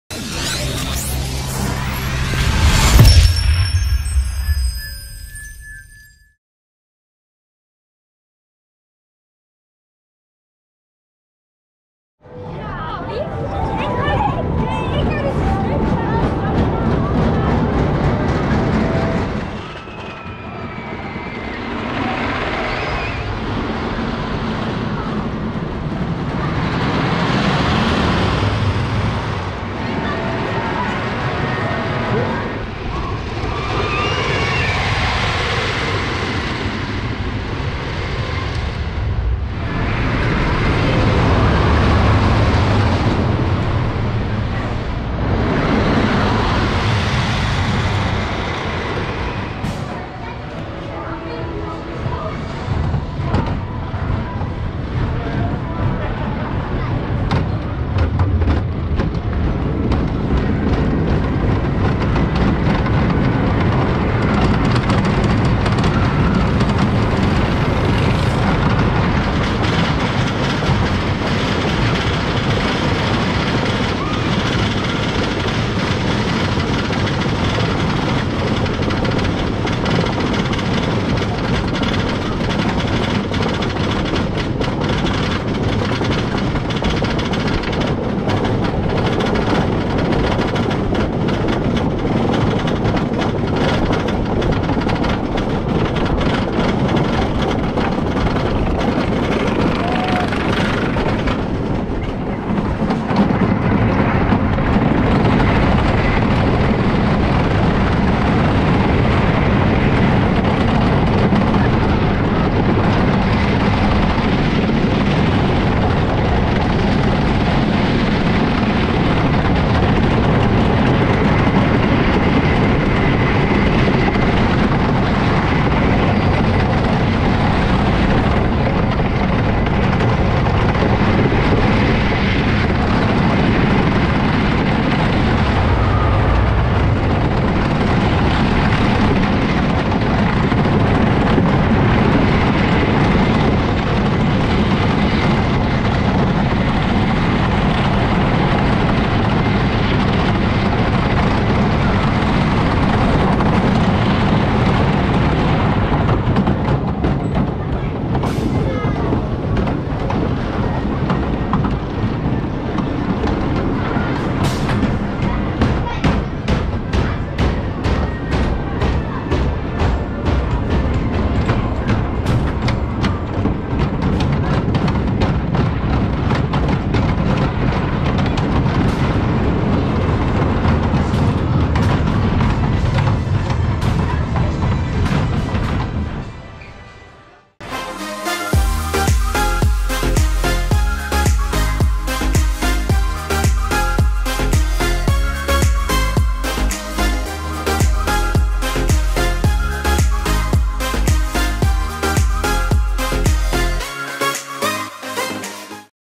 Troy 4K Front Seat POV - Toverland
Take a ride on Troy which is located at Toverland, Sevenum, Netherlands.

- XtremeCoasters Network -